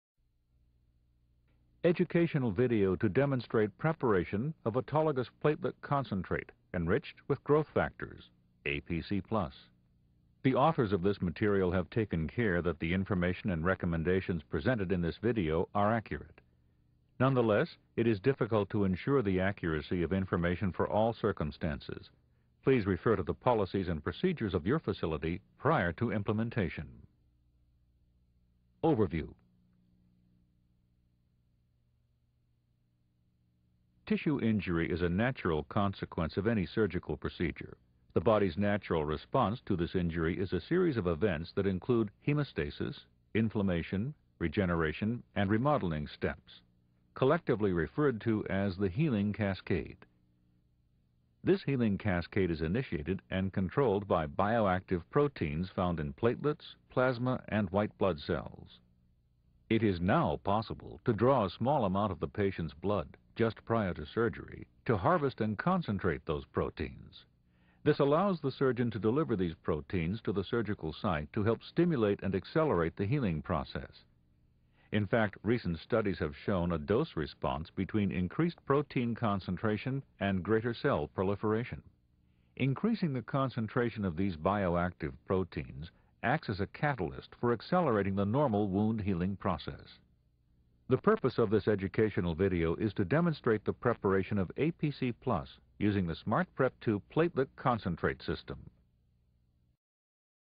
Platelet Rich Plasma - PRP - Dr. Bill Johnson
Dr. Bill Johnson and Innovations Medical is the 1st practice in Dallas Fort Worth to offer PRP Fat Transfer, of Activated Fat Transfer.  This medical breakthrough allows for a substantially higher fat cell survival rate than with previous technologies.